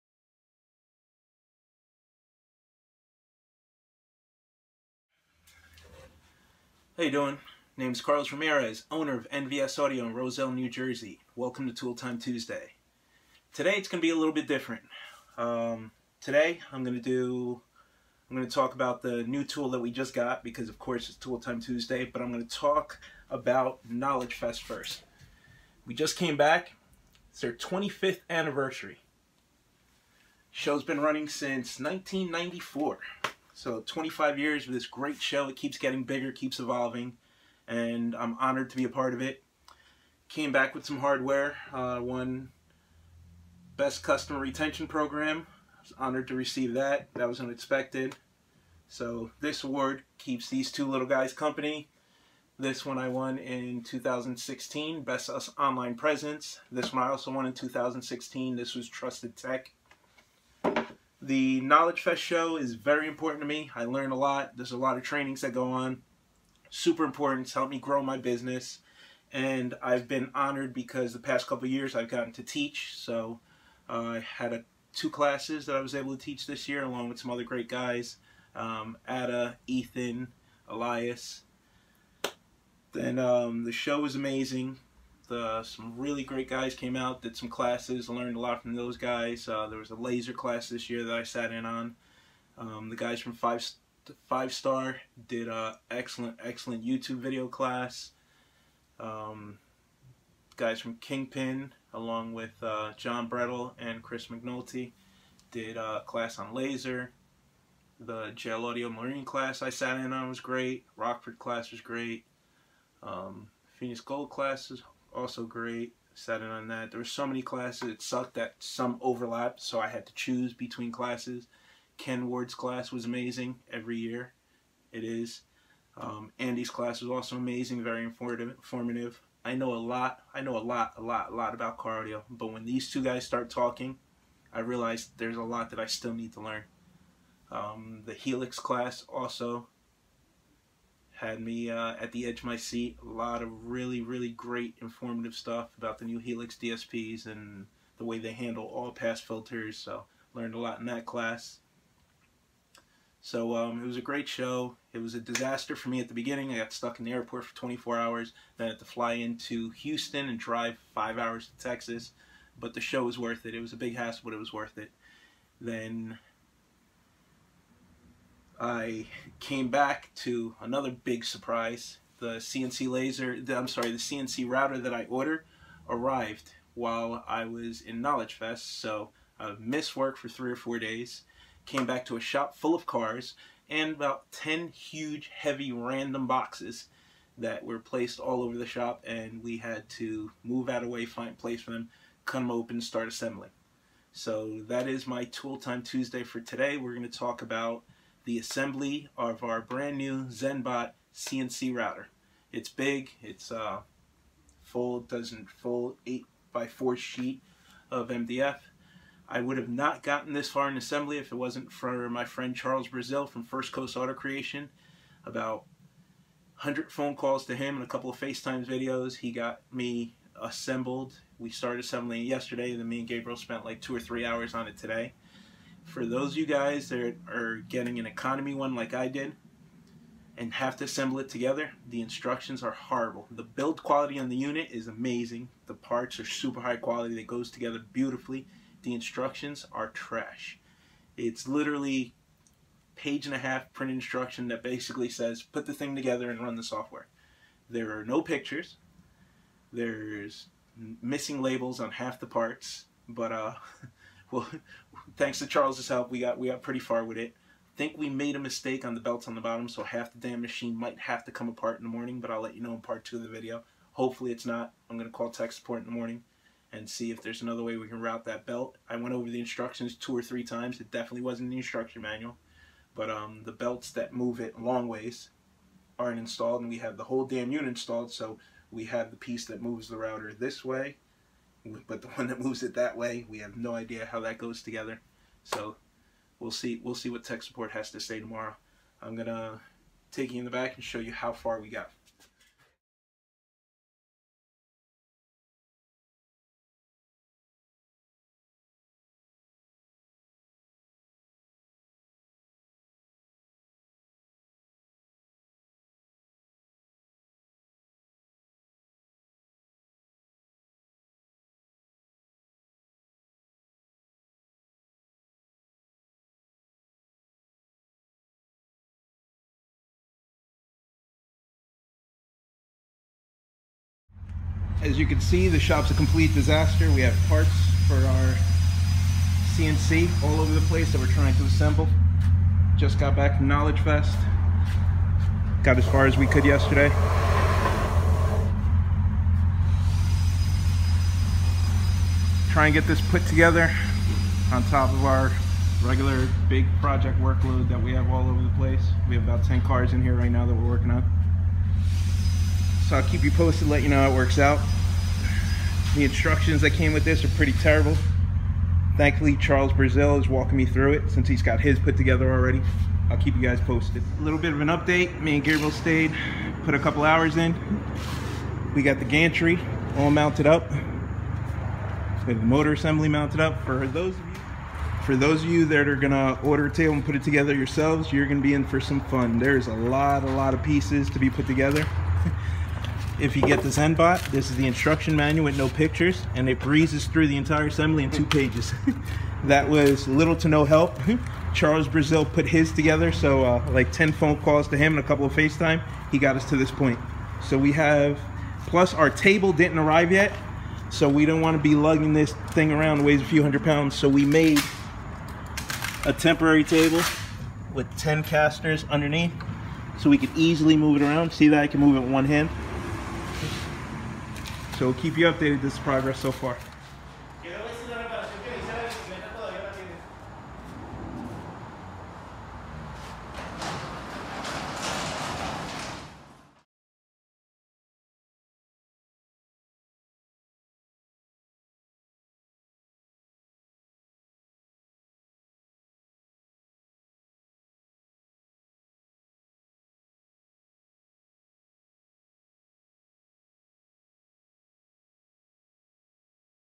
Tooltime Tuesday knowledgefest review and Zenbot cnc router
It's tool time Tuesday.
I get a lot of questions about the tools I use here at my shop so I decided to make a weekly post to share .

Today we talk about the zenBot 4896 4x8 cnc router